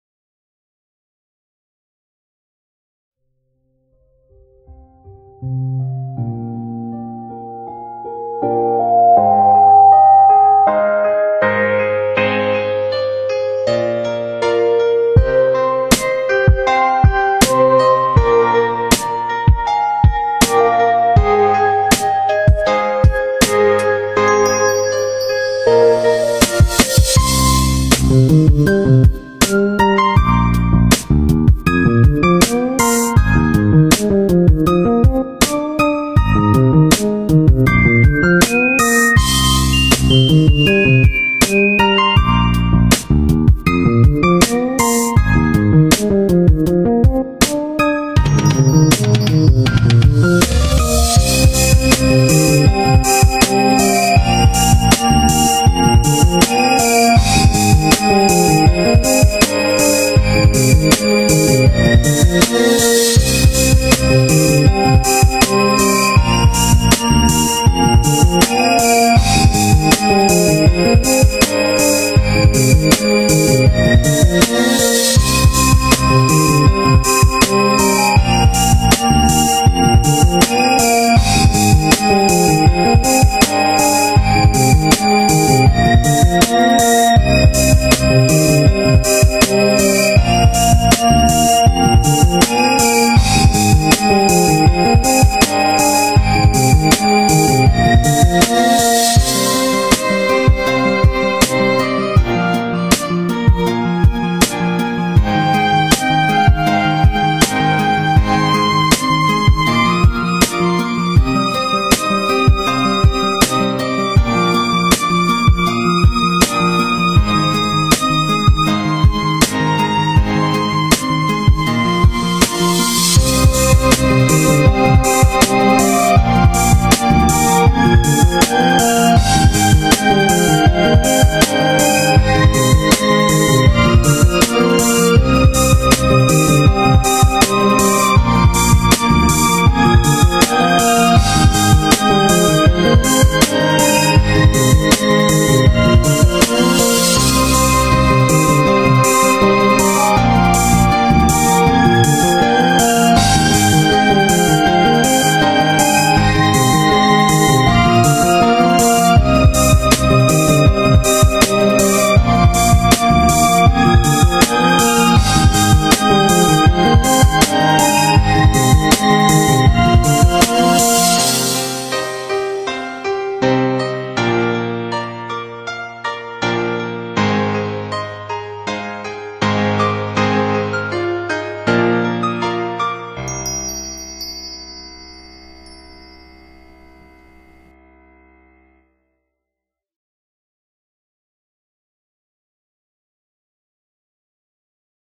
Sam Sullivan - Beautiful Piano String Rap Beat
This is a beautiful beat I made in about two days. Sorry I havent been posting for a while.